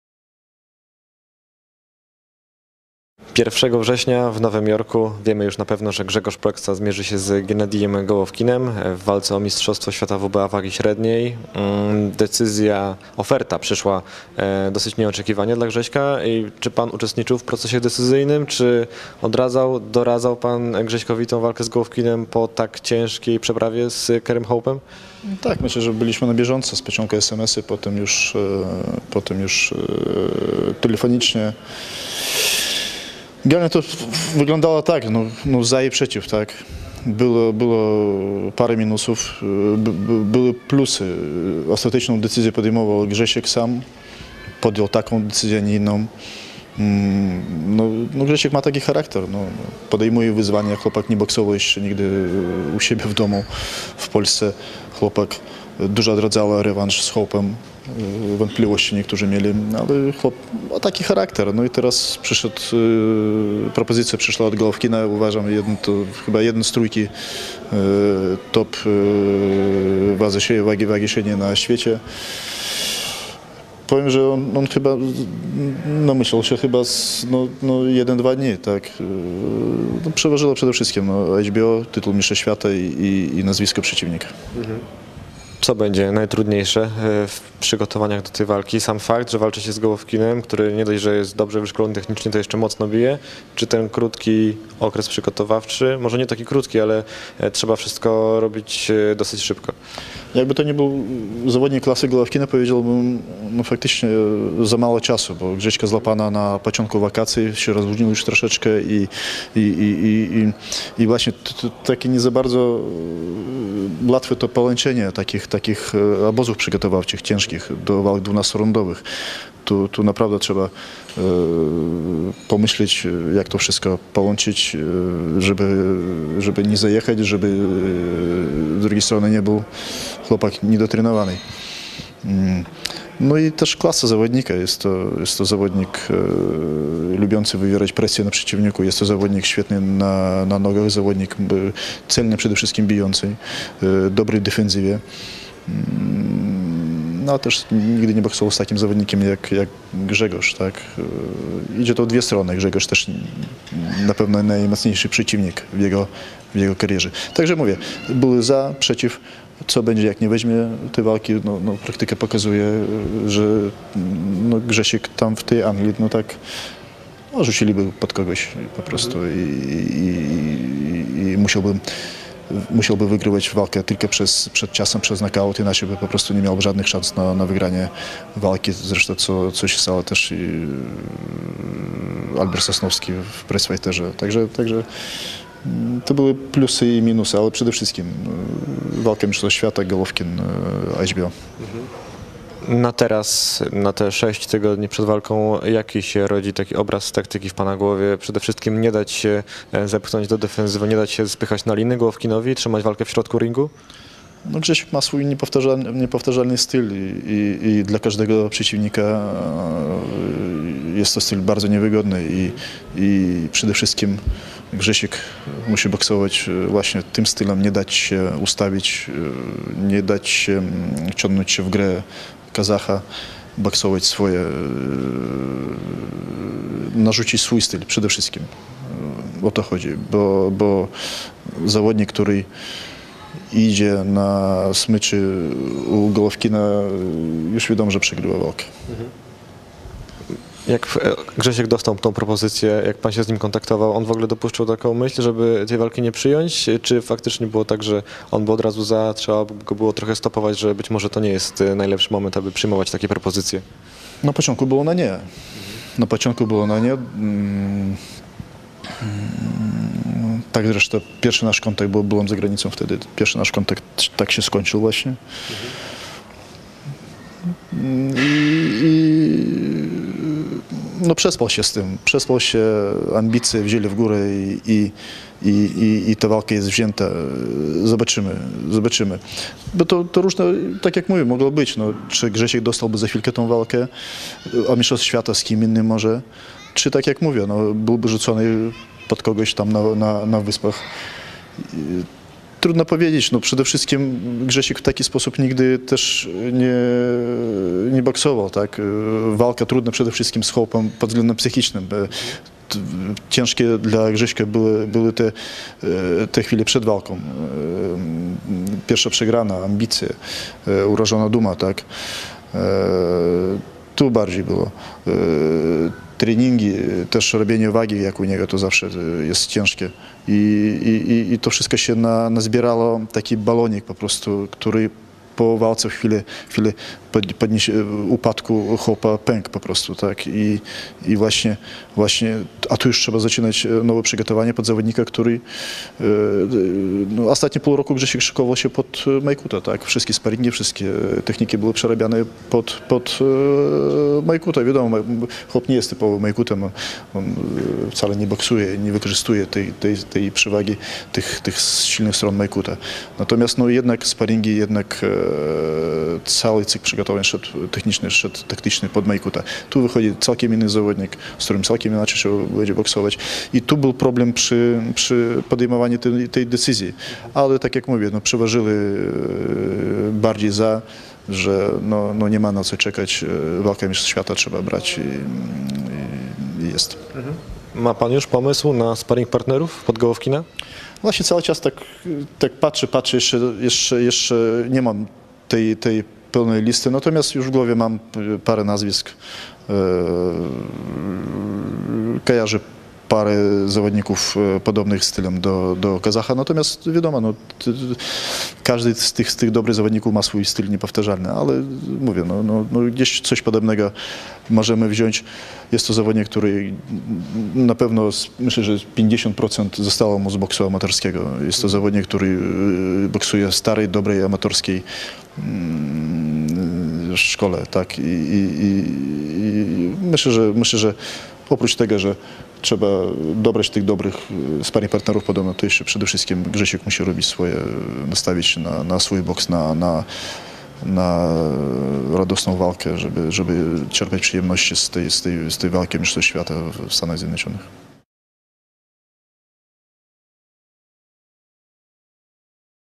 Fiodor Łapin o walce Proksa - Gołowkin
Trener Fiodor Łapin o walce Grzegorza Proksy z Gienadijem Gołowkinem o mistrzostwo świata WBA wagi średniej.